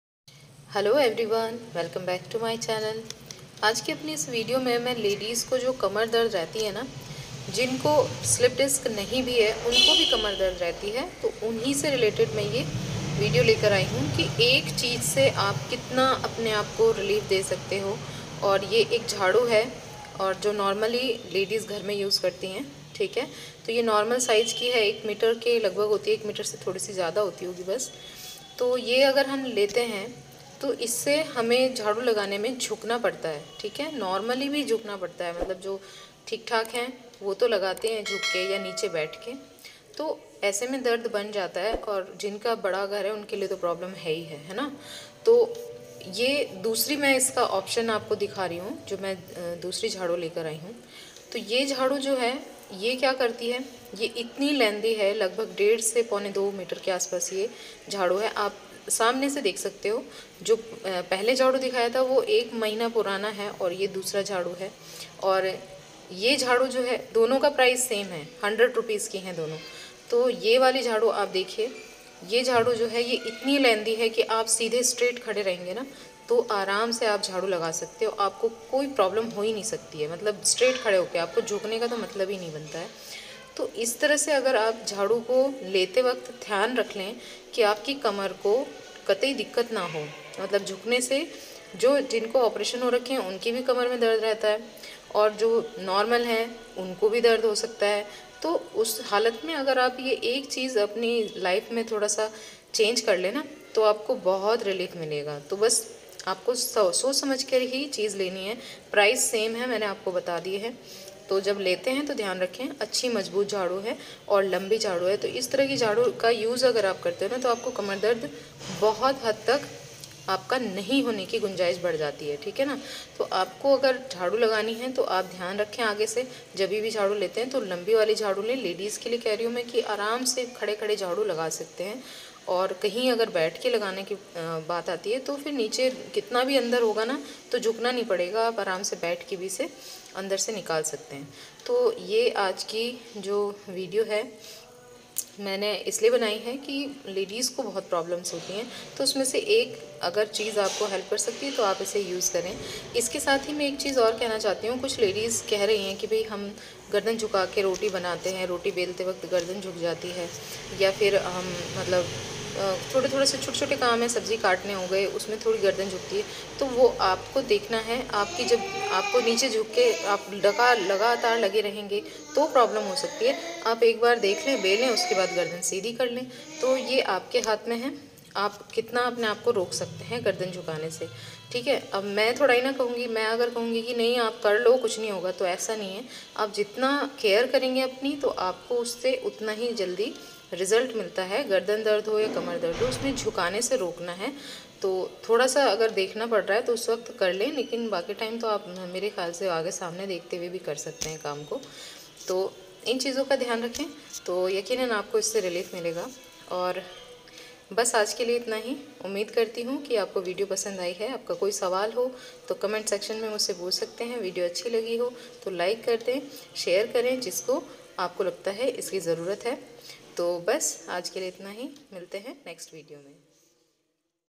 क्या एक चिज़ कमर दर्द को होने से रोक सकती है।||L4-L5-S1 कमर दर्द, स्लिप डिस्क, problem
online long jhadoo:---